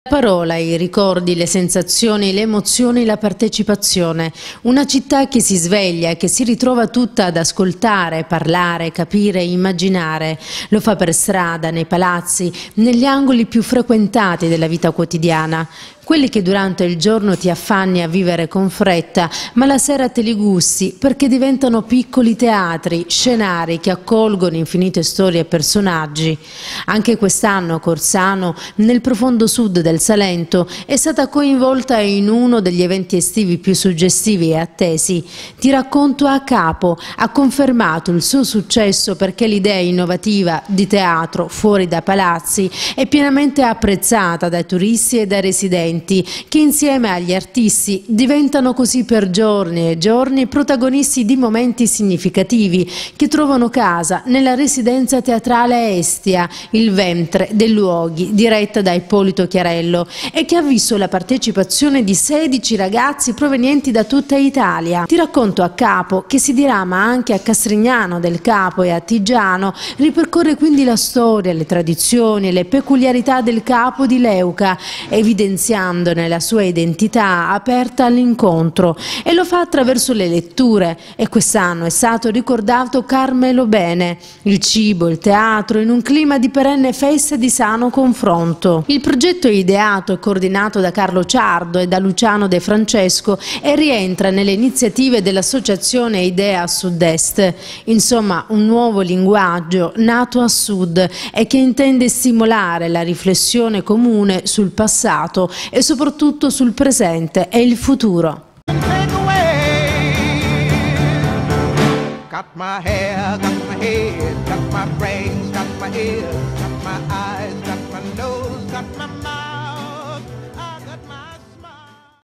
Nuovo successo per Ti racconto a Capo, quando il teatro rende tutti protagonisti
Rinnovato successo per uno degli eventi più suggestivi dell'estate salentina. TI RACCONTO A CAPO anche quest'anno ha saputo coinvolgere residenti di Corsano, salentini, turisti e artisti provenienti da tutta Italia nel progetto che porta per strada il miglior teatro, quello che fa riflettere. L'evento è ideato e coordinato a Carlo Ciardo e Luciano De Francesco, è organizzato dall'Associazione IDEE A SUD EST, dall'Assessorato alle Politiche Giovanili e Associazionismo della Città di Corsano, da NASCA Teatri di Terra e diverse collaborazioni.